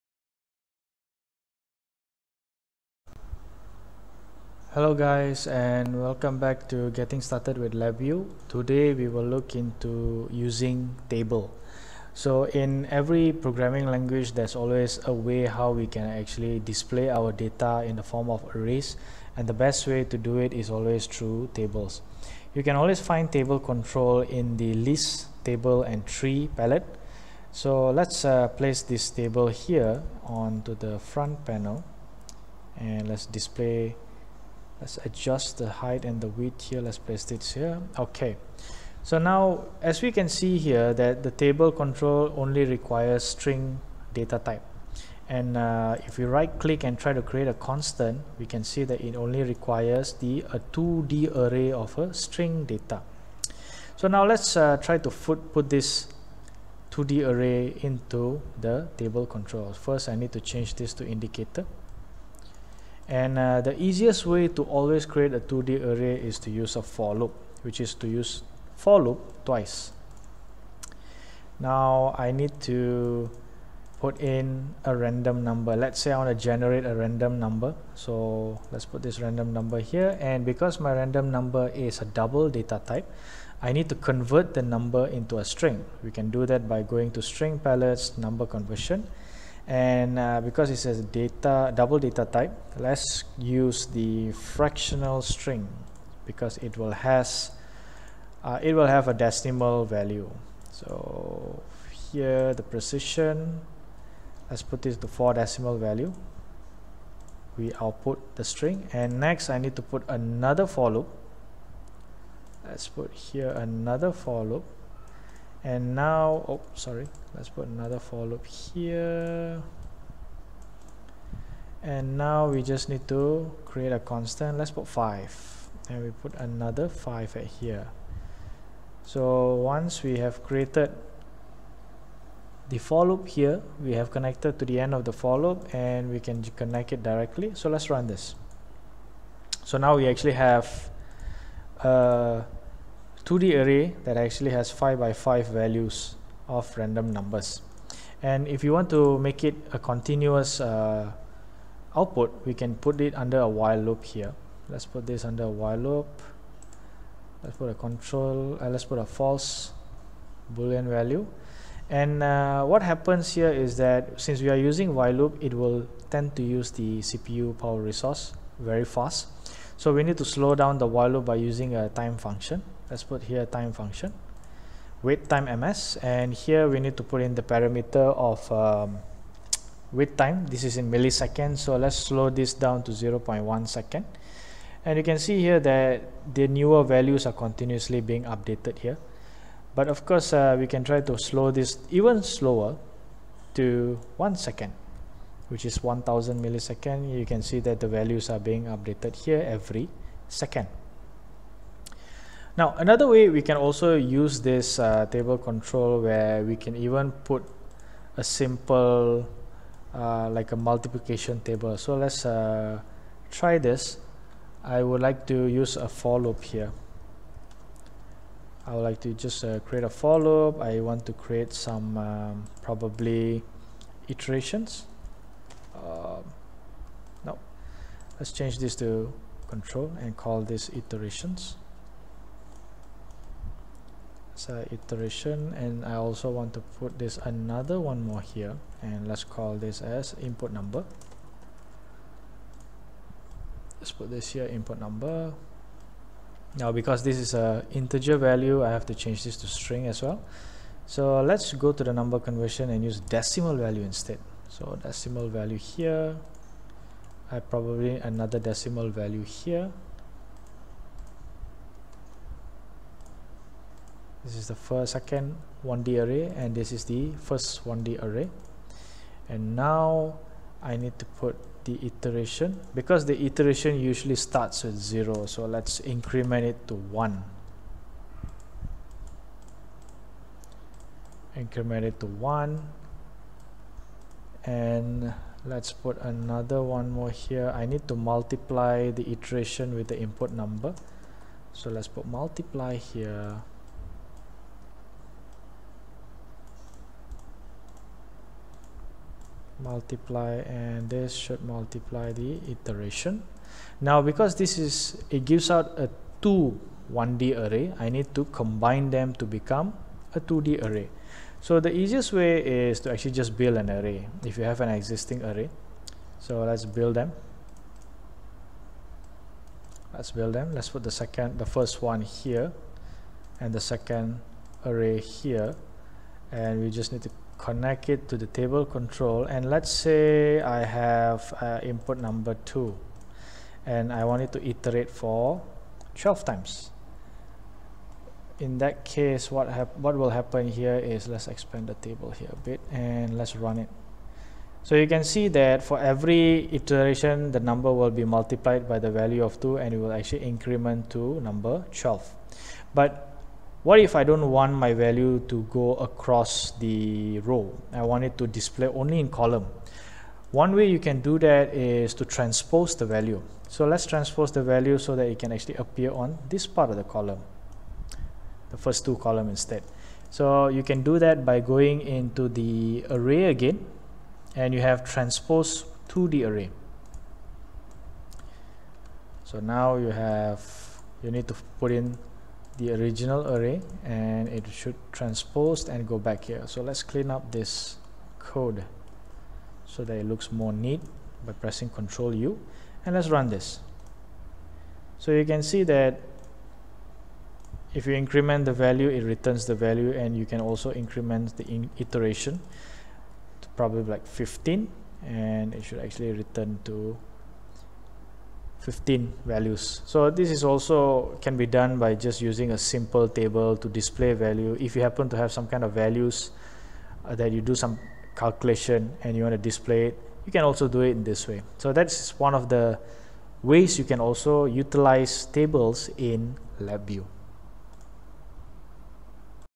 Using Table in LabVIEW Part 1
This video shows how to use Table control in LabVIEW by creating a 2D string array as input values. The examples explains the basic structure of the table and uses two examples;

1. Create 2D array in For Loop
2. Build 2D array from a For Loop